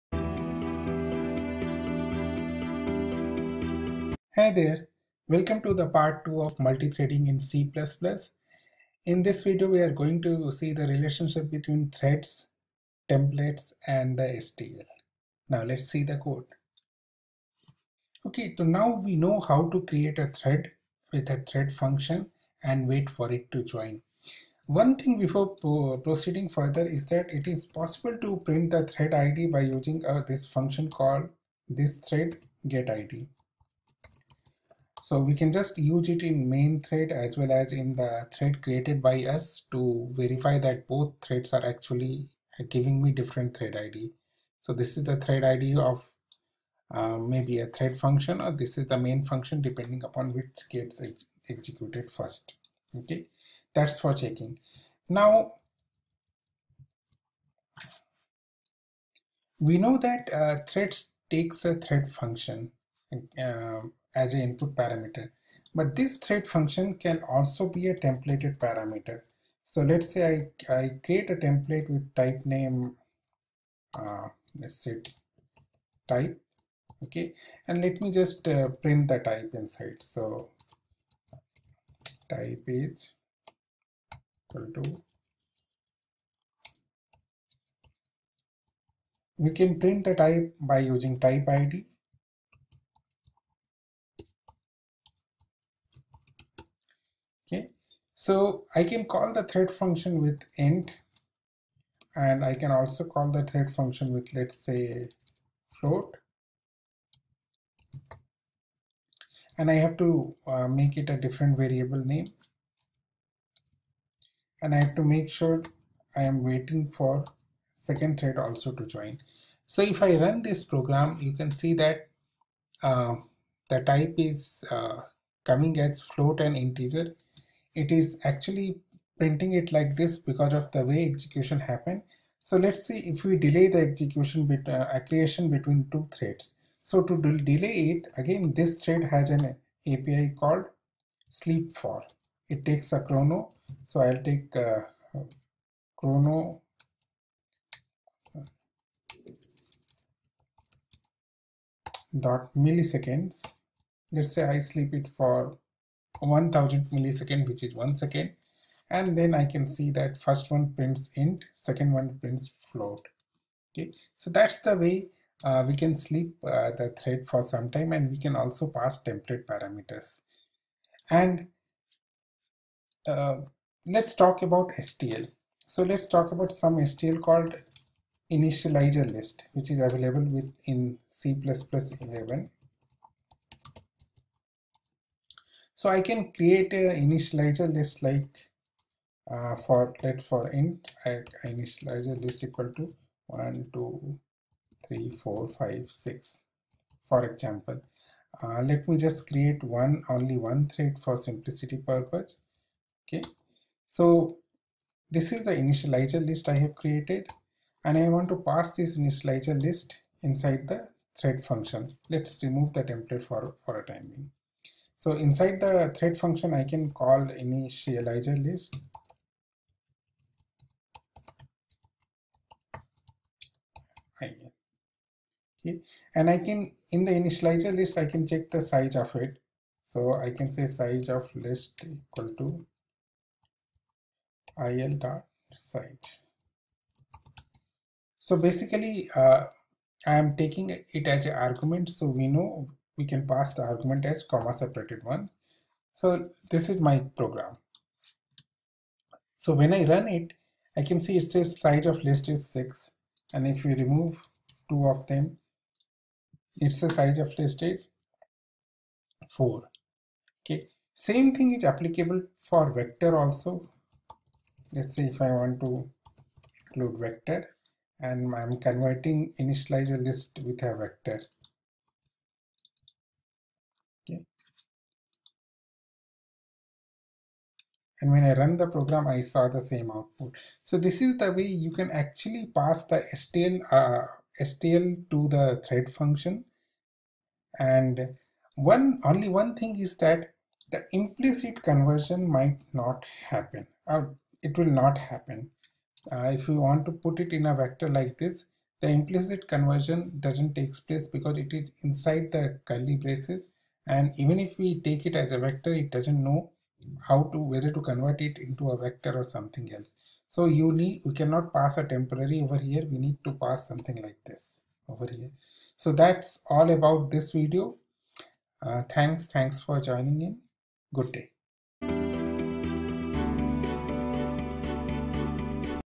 C++ Multithreading Part- 2 :Creating c++ Threads With Templates & STL (standard template library)
👉 Channel Name changed because of Rebranding Exercise. Existing Social media handles and links are no longer valid

Welcome to the video series on Multithreading in C++ programming Language. From C++11 onwards multitheading is in built into the c++ and we don't need to call posix threads and link a separate library for the same.
with c++ multithreading, we can not only create threads but can also handle  the concurrency with mutex and locks. In today's multicore multi CPU environment, Multithreading is one of the best way of harnessing the available hardware resources.

C++11 has introduced native threads as part of C++ library which not only allows us to create the threads, but also use the functional programming aspects of C++ within the threading mechanism.

This video is part of a series where I've touched upon how we can make use of C++11 constructs to create and manage threads efficiently and effectively.

This is part - 2 which demonstrates how to use of threads (std::thread) with templates and STL (Standard Template Library)

Hope it helps you in learning something new.. enjoy!